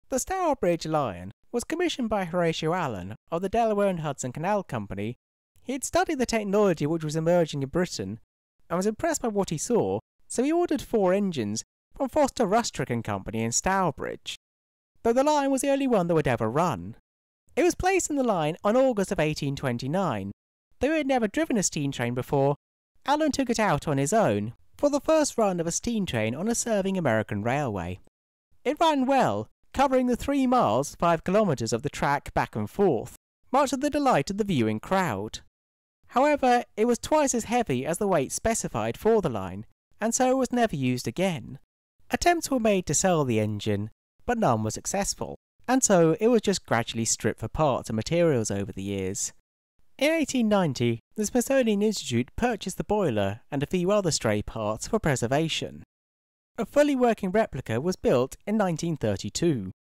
The Stourbridge Lion - American Steam Power's False Start
This is the story of the Stourbridge Lion, the first steam train to run on a serving American railway.